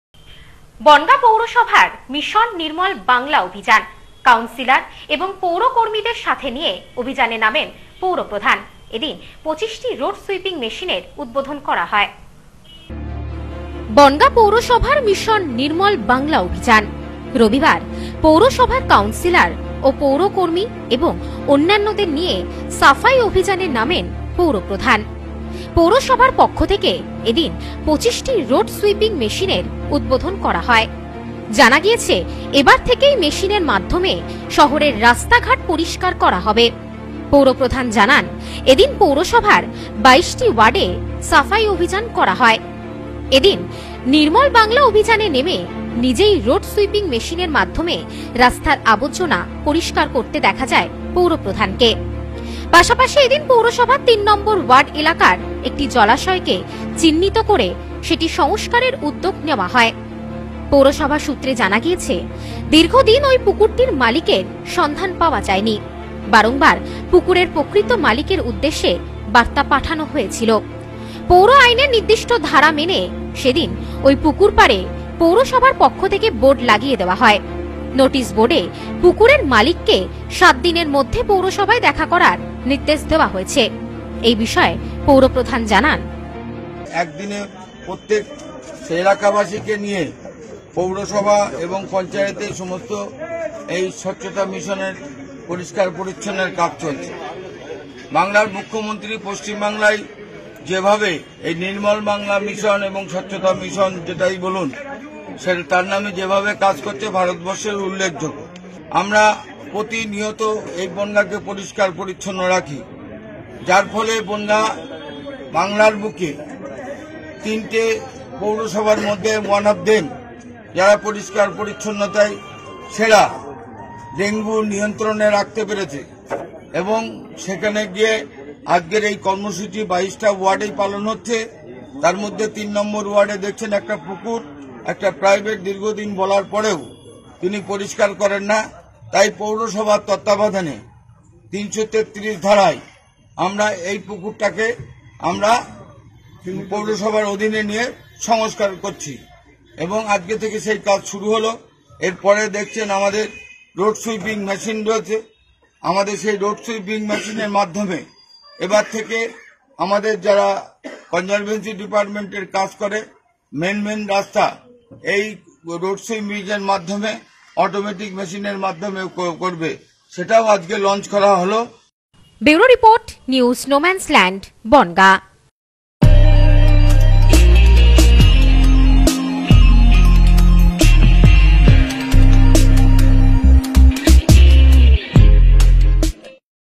বনগাঁ পৌরসভার মিশন নির্মল বাংলা অভিযান।
বনগাঁ পৌরসভার মিশন নির্মল বাংলা অভিযান। কাউন্সিলার ও পৌরকর্মীদের নিয়ে অভিযানে নামেন পৌরপ্রধান। এদিন উদ্বোধন করা হয় ২৫ টি রোড সুইপিং মেশিন।